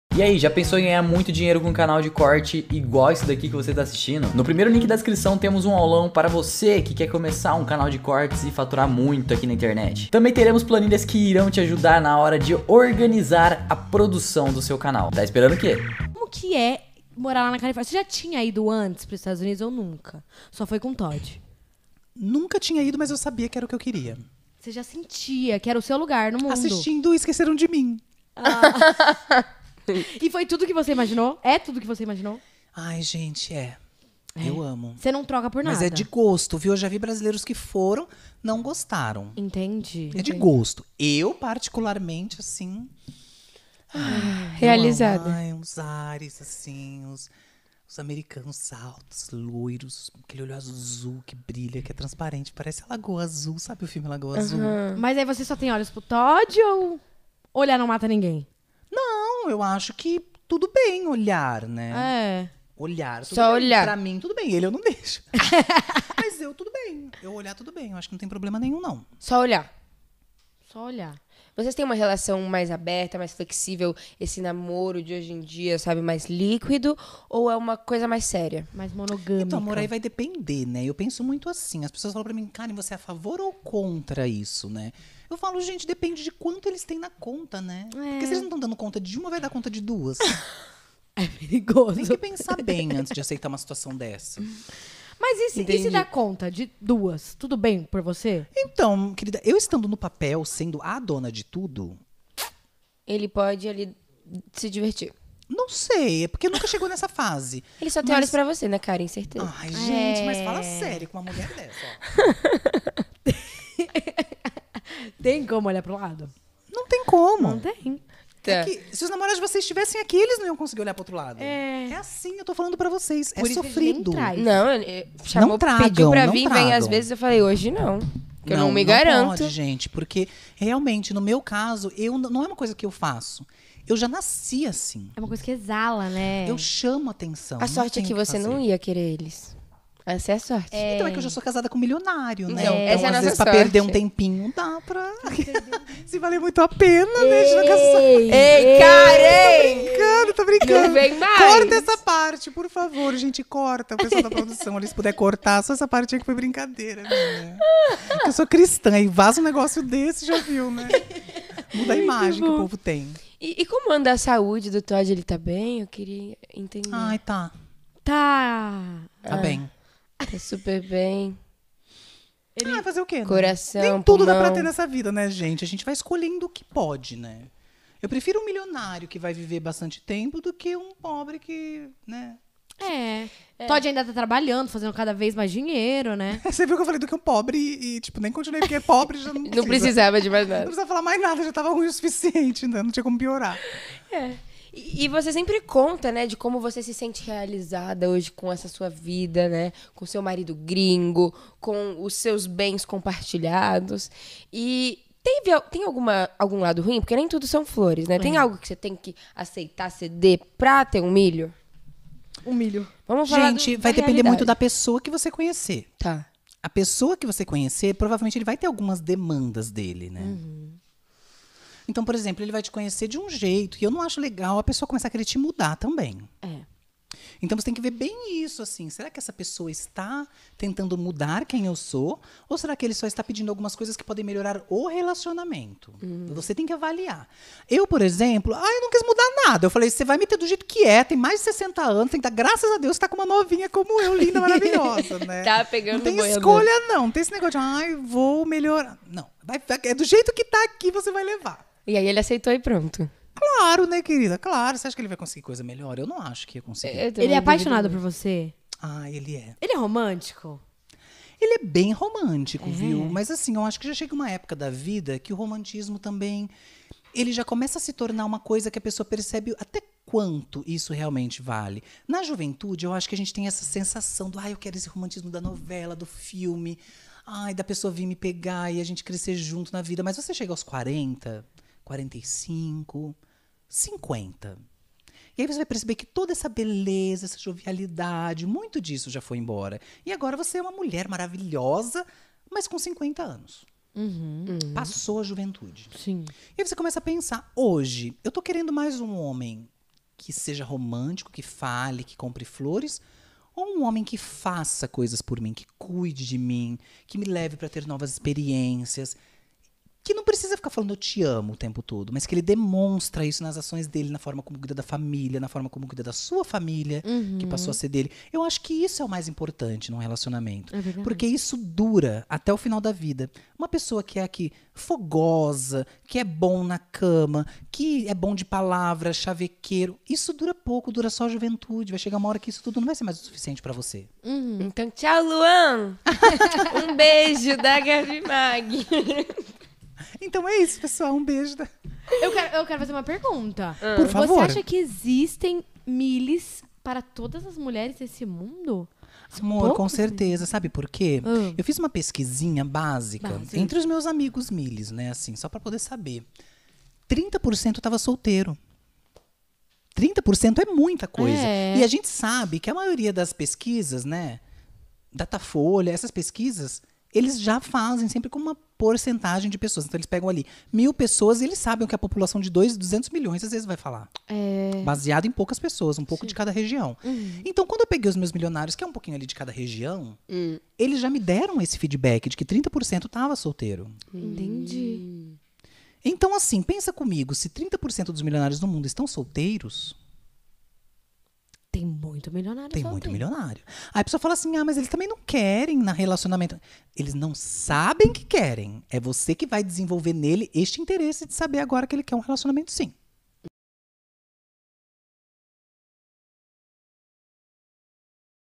🔴 O TOD TEVE QUE ACEITAR O MEU JEITO DE SER - KAREN KARDASHA | + um corte visse as
🔴 + UM CORTE VISSE

KAREN KARDASHA fala do seu RELACIONAMENTO com TOD KARDASHA, e conta a HISTÓRIA de como se conheceram.